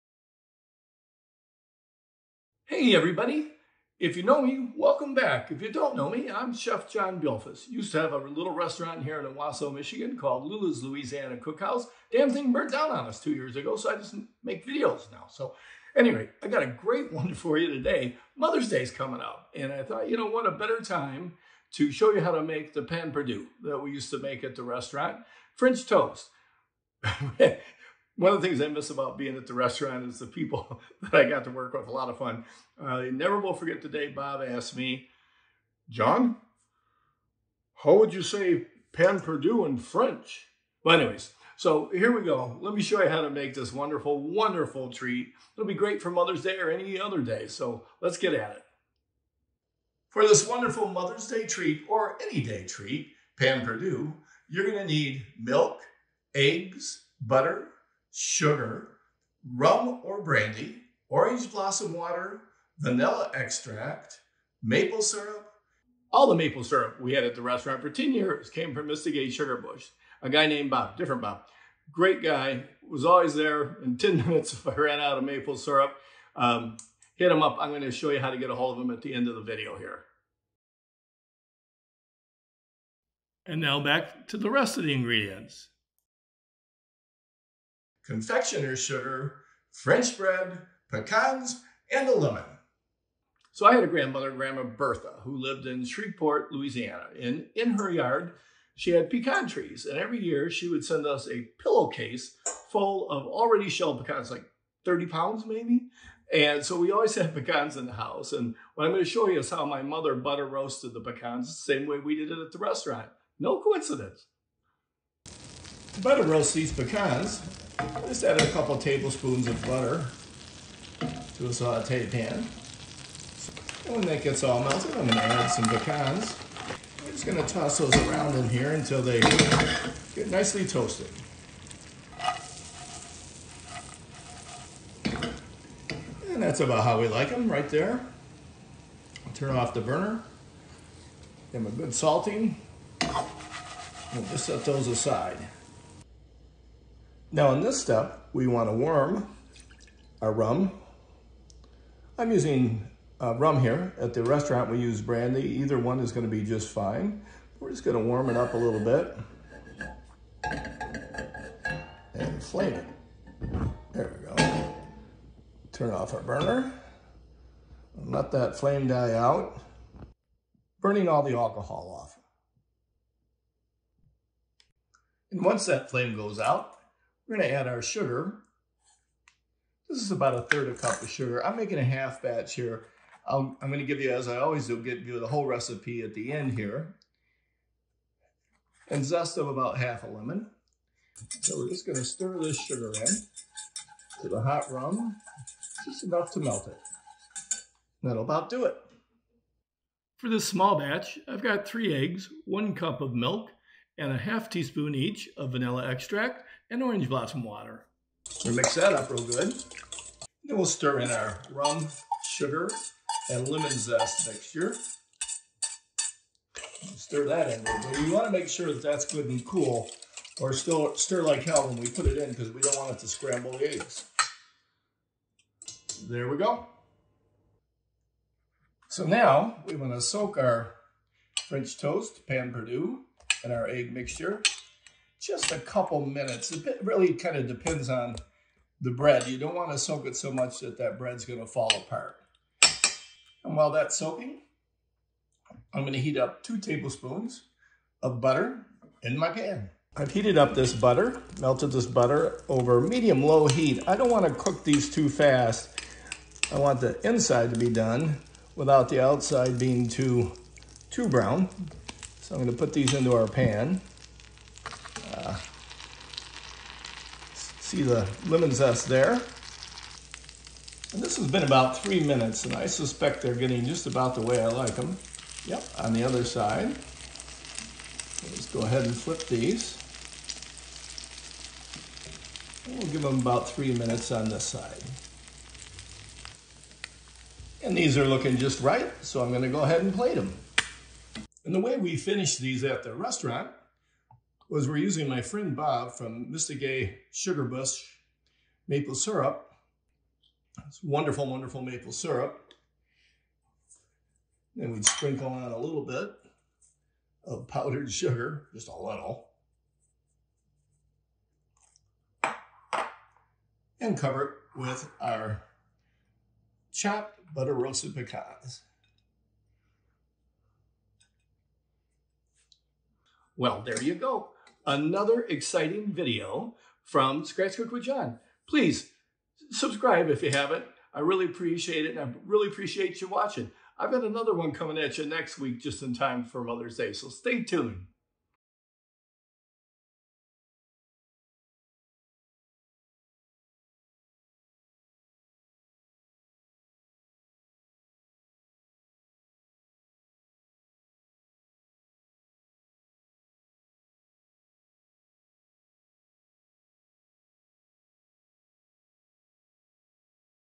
How to make Pain Perdu for Mother's Day - Fancy French toast from Chef John!
Chef John shows you exactly how to make Pain Perdu as served at multi-award-winning Lula's Louisiana Cookhouse!